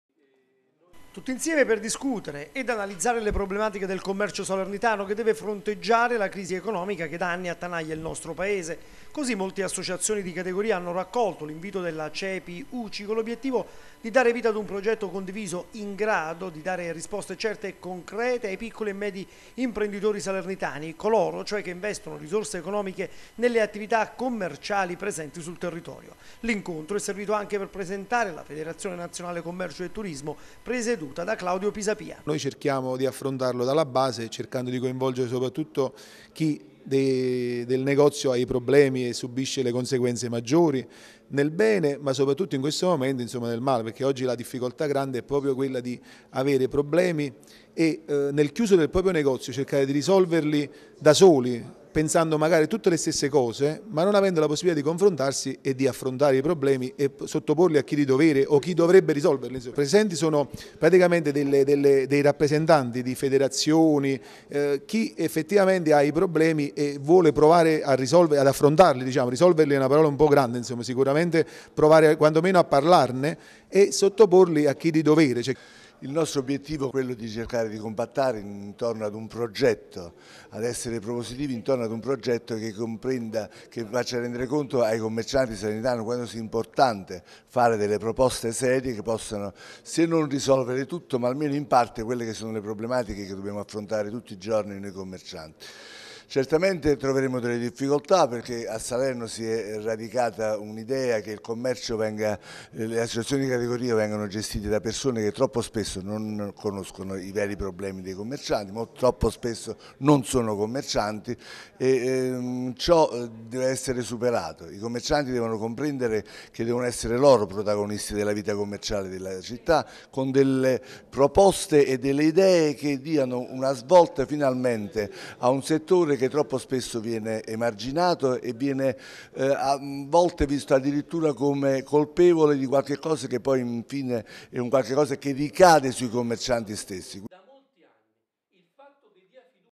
Commercio a Salerno, la Cepi-Uci prova a mettere insieme le varie associazioni
Fare rete tra le associazioni con l'obiettivo di provare a risolvere alcuni dei problemi del commercio salernitano. A chiederlo è la Cepi-Uci che stamani ha convocato i rappresentanti di altre associazioni presenti sul territorio con l'obiettivo di dare vita ad un progetto condiviso. (090315)